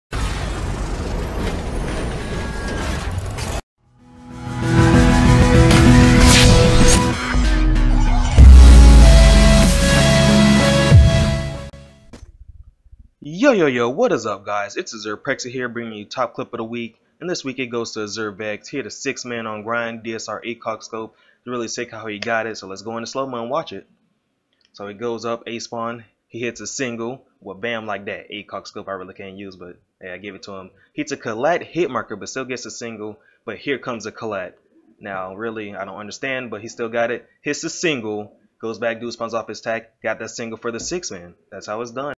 AzuRe Clip Of The Week ft. AzuRe Vex
Clip of the week goes to AzuRe Vex for hitting a 6man with the dsr acog scope on Grind!